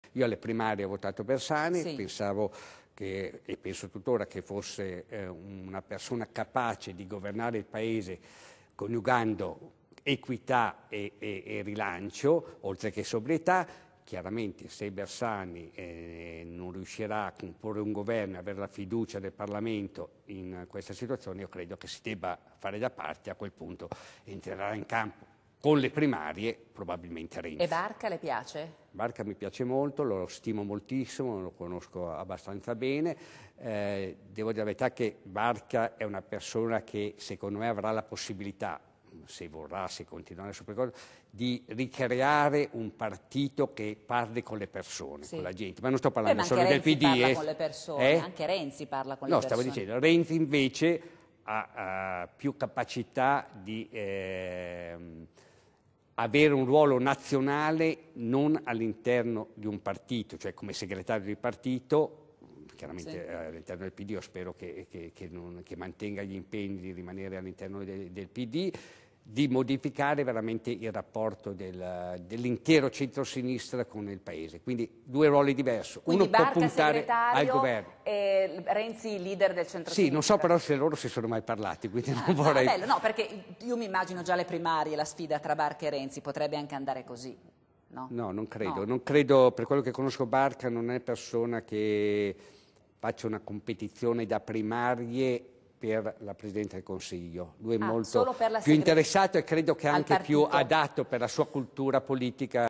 Pisapia: se Bersani fallisce dopo tocca a Renzi
Intervista al sindaco di Milano durante Linea d'Ombra del 16 aprile 2013